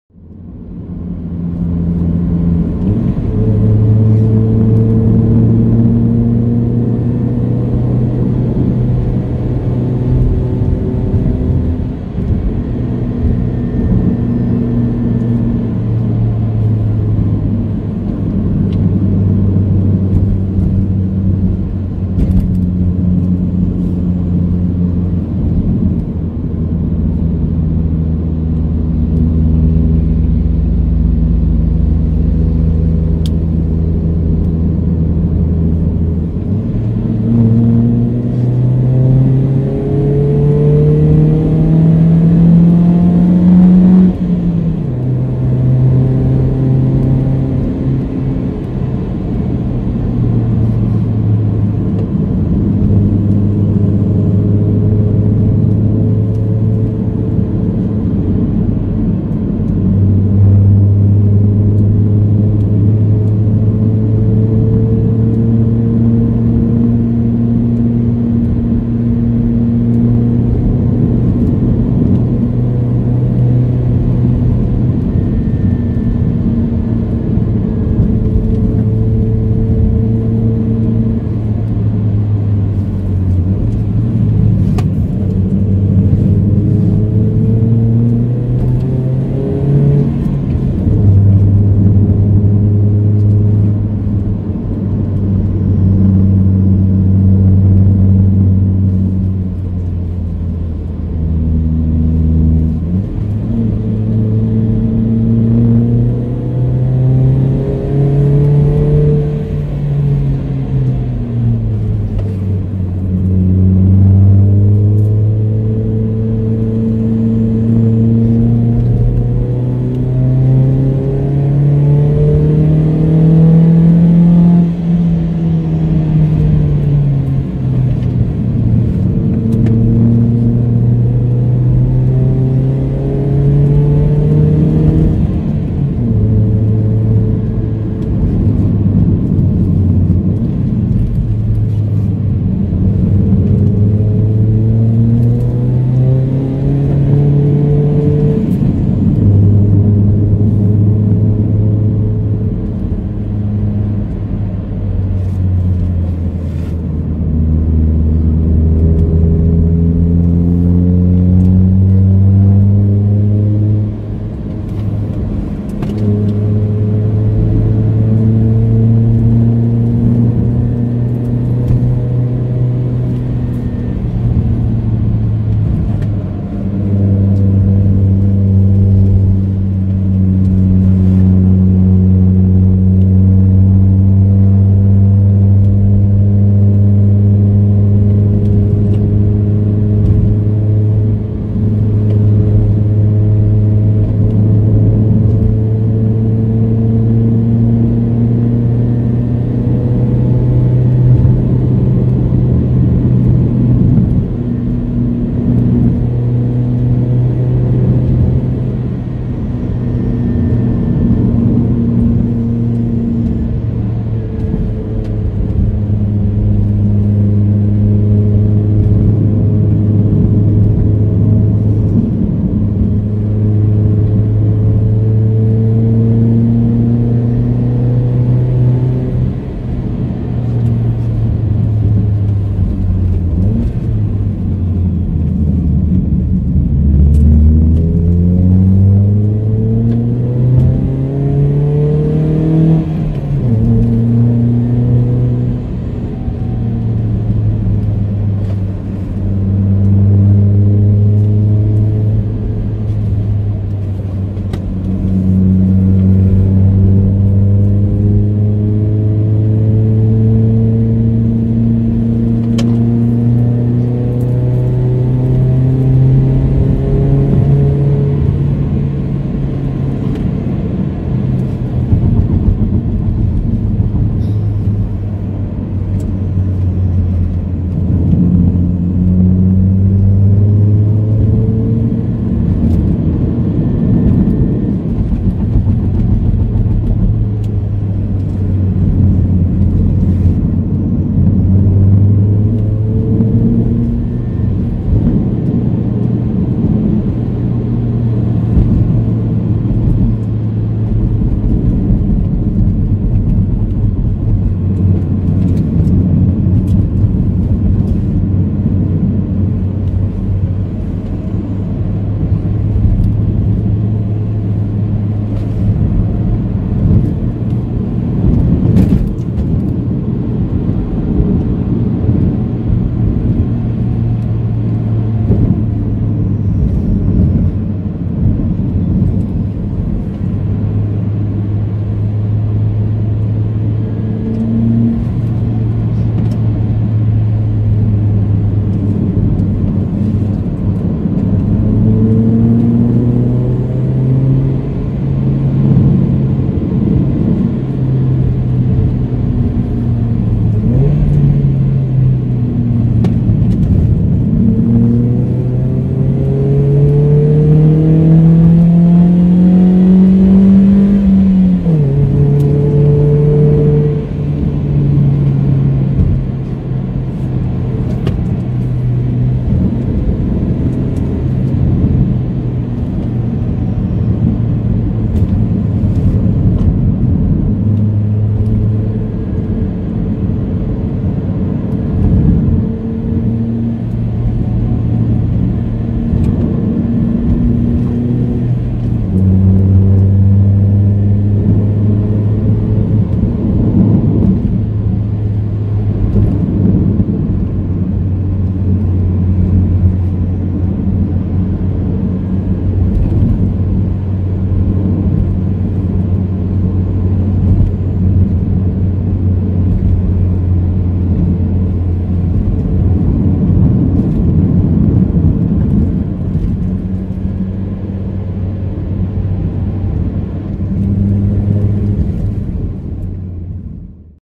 TrackAddict + RaceRender | Inspira 1.8(M) Genting
Recorded using TrackAddict App, further edited with RaceRender By HP Tuners. Night casual Genting uphill run.